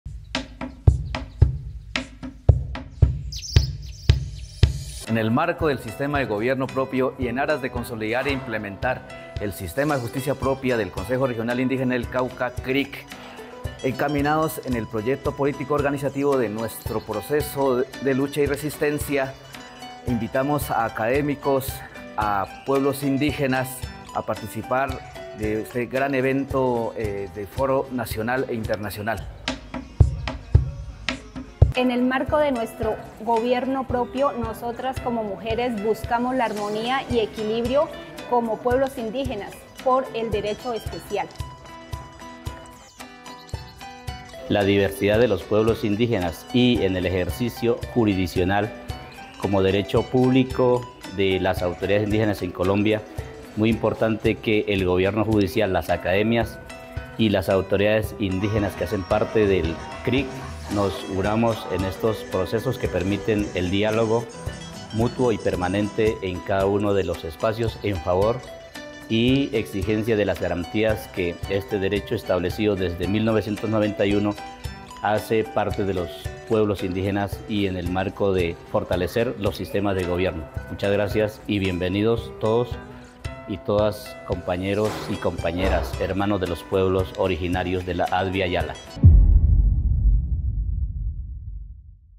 Foro internacional interjurisdiccional de pueblos indígenas CRIC
Foro internacional interjurisdiccional de pueblos indígenas en el  marco de la construcción de la ley de coordinación, entre la jurisdicción especial indígena y el sistema judicial nacional